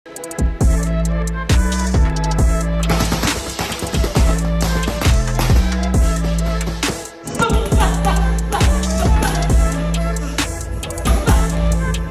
16-year-old amateur boxer Javon Walton is the youngest Jordan brand athlete ever | NYPSports
Euphoria actor and 16-year-old amateur boxer Javon Walton is the youngest Jordan brand athlete ever.